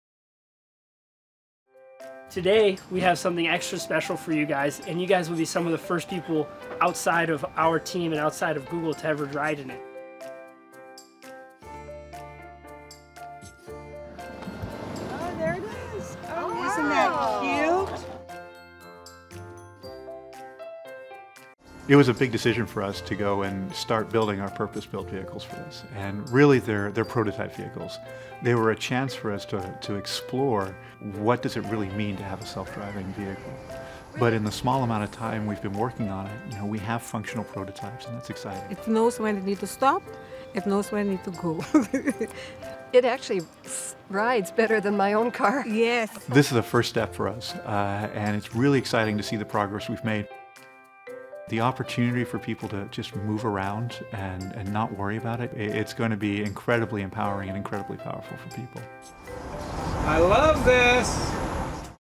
Google unveils prototype of its very own self-driving car
Google unveiled plans to build its own self-driving car -- minus the steering wheel -- that it hopes to begin testing in the coming months. For Google, the car is to pioneer individual transport that needs only a stop-and-go function. Duration: 01:02